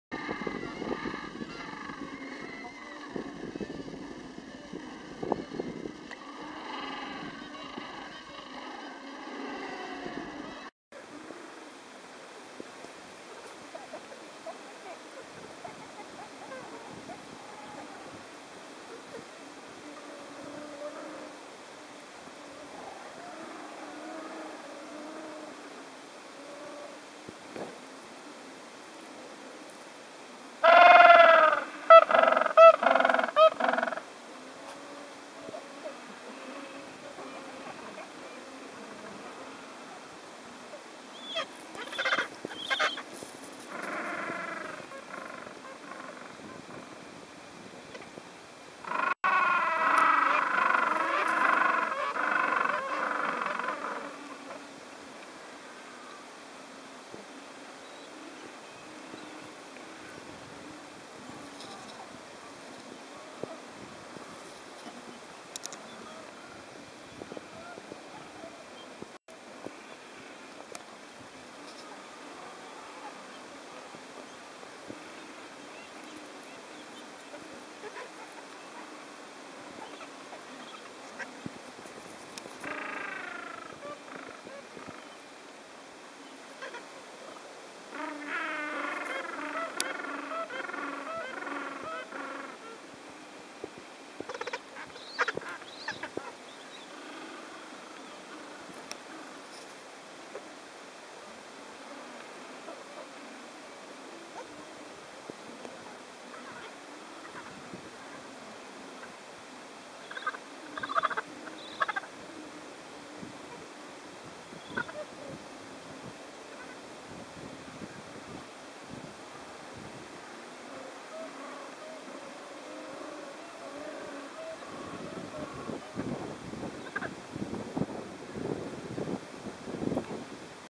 Cuverville 2012-02-16
A short clip from visit with the Bark Europa in 2012 to Cuverville Island in Antarctica